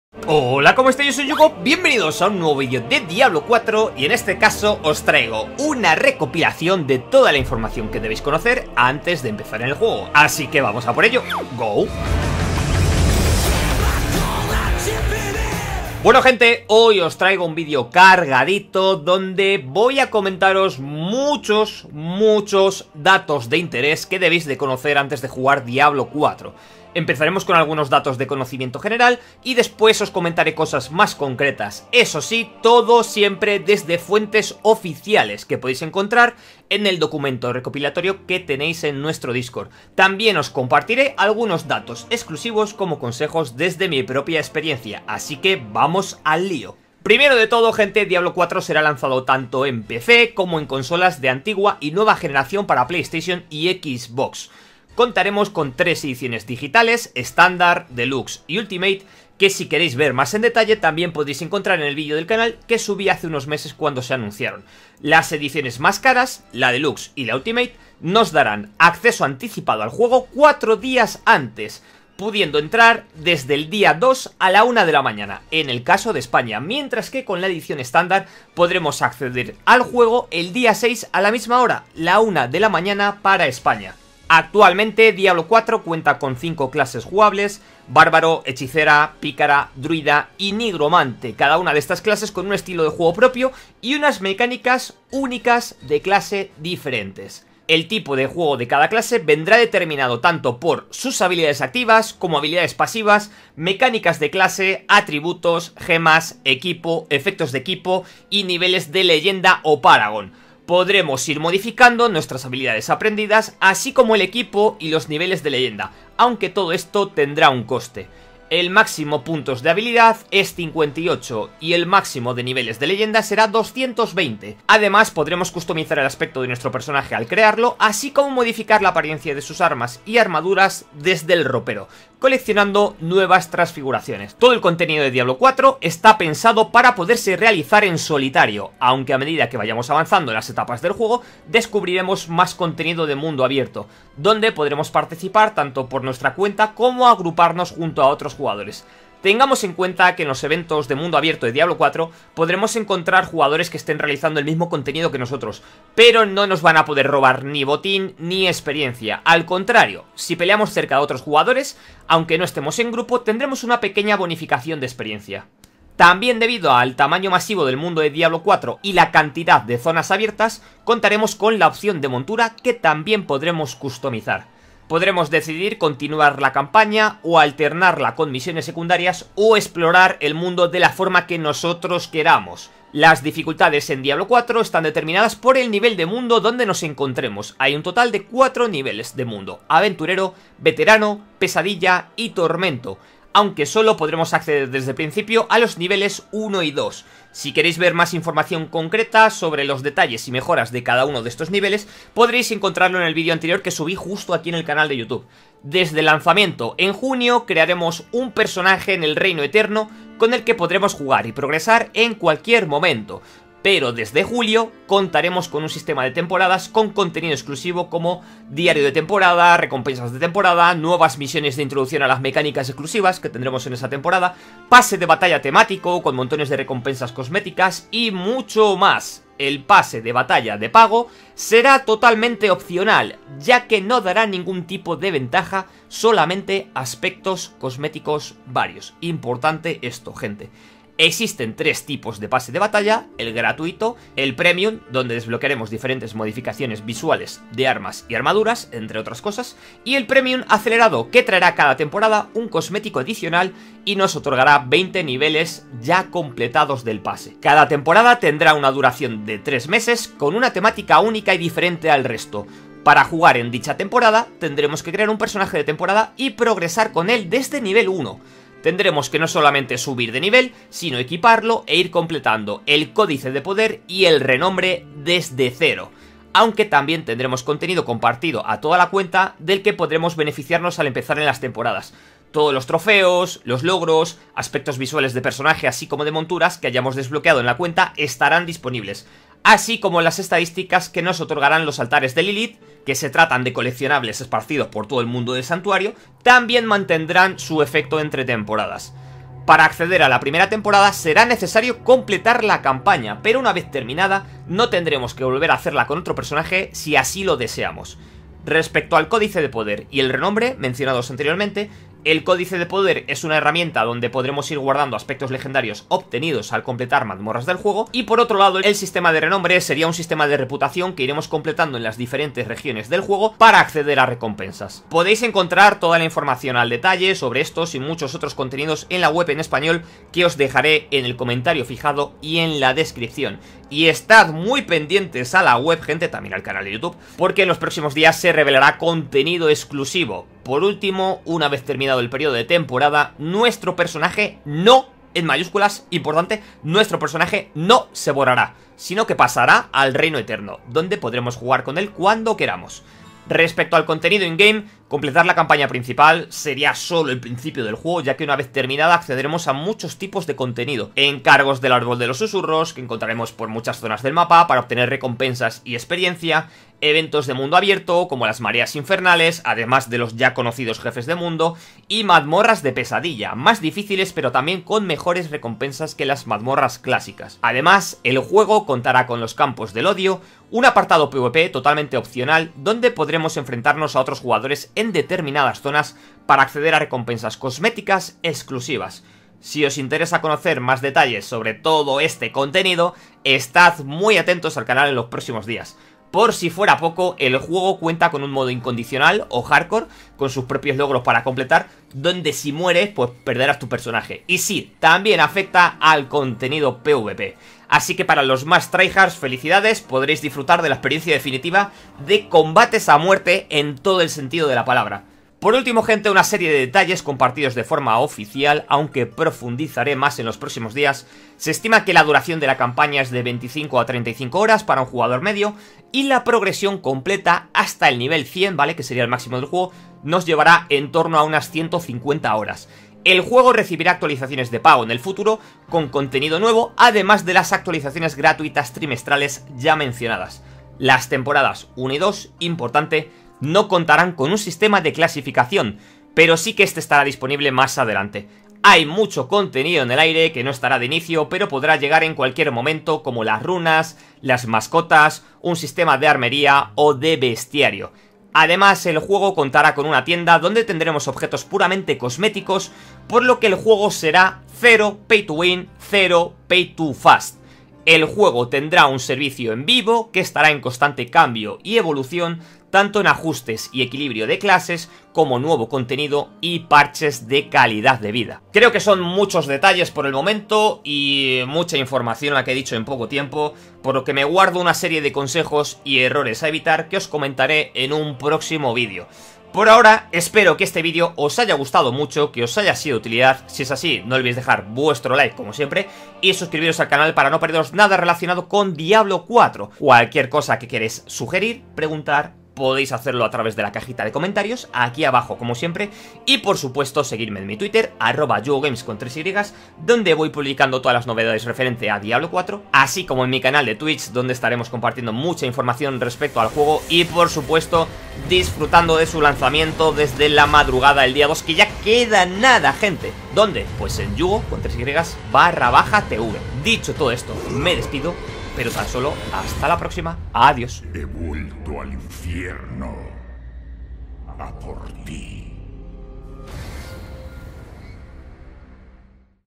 COSAS QUE DEBES SABER ANTES DE JUGAR | DIABLO IV ESPAÑOL
Hoy os traigo una recopilación de toda la información que debéis conocer antes de jugar Diablo IV. Sistemas de progresión, contenido end-game, temporadas, monetización, planes a futuro.

Noticias e Información sobre Diablo IV, el MMO de Riot, Throne and Liberty,  y otros grandes lanzamientos del mundo MMORPG y ARPG.